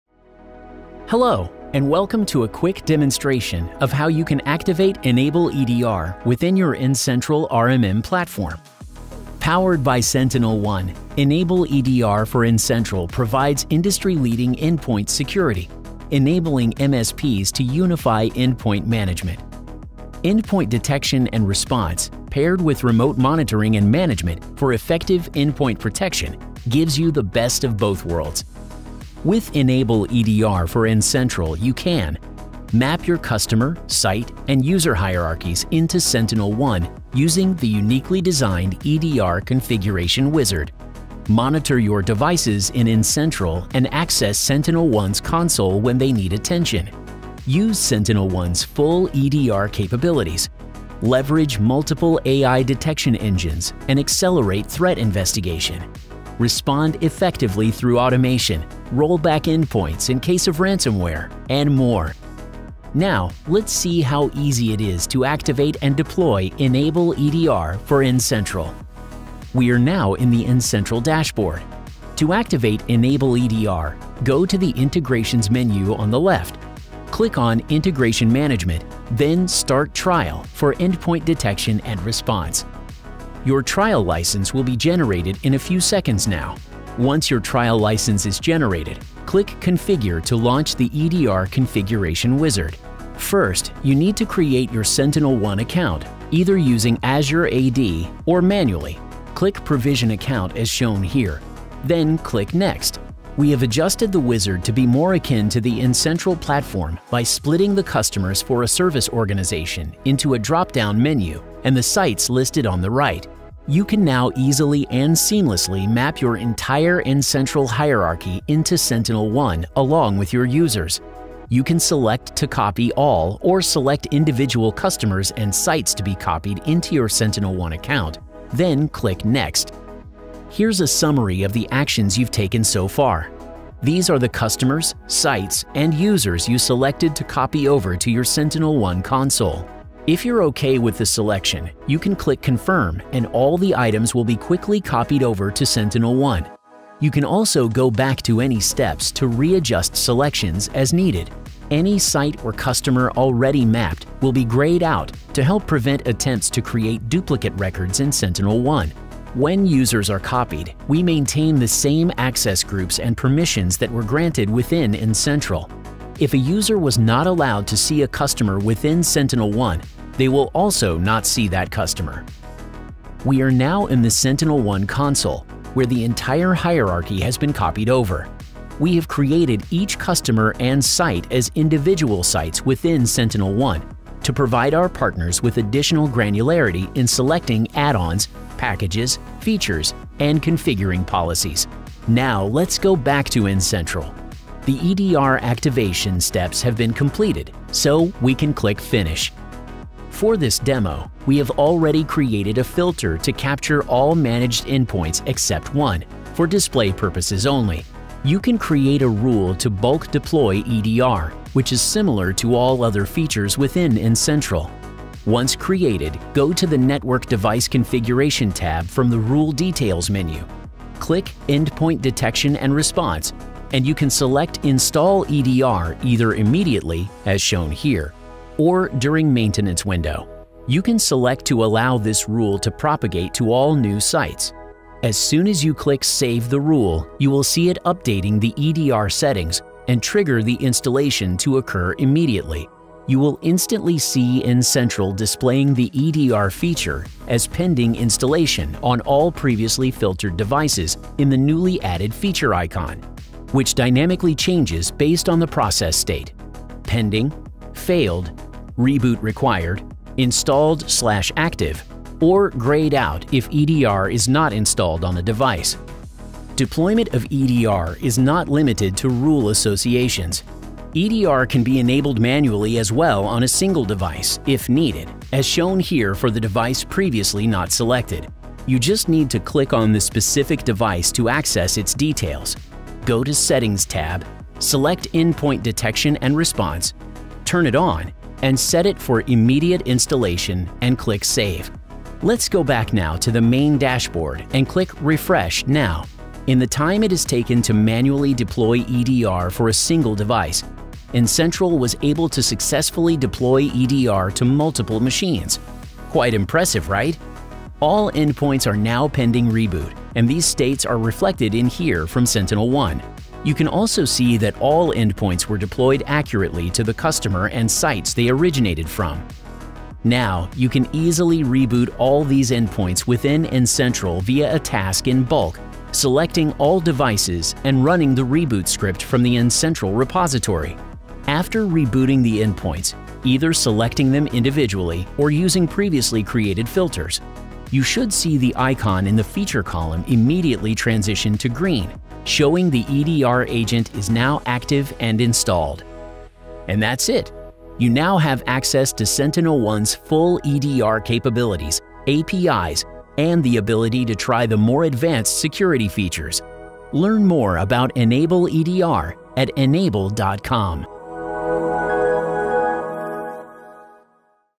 N-able EDR for N-central: Get Started
Powered by SentinelOne, N-able EDR for N-central provides industry-leading endpoint security, enabling MSPs to truly unify endpoint management and get the best of both worlds: endpoint detection and response paired with remote monitoring and management for effective endpoint protection. See how easy it is to activate and deploy N-able EDR for N-central.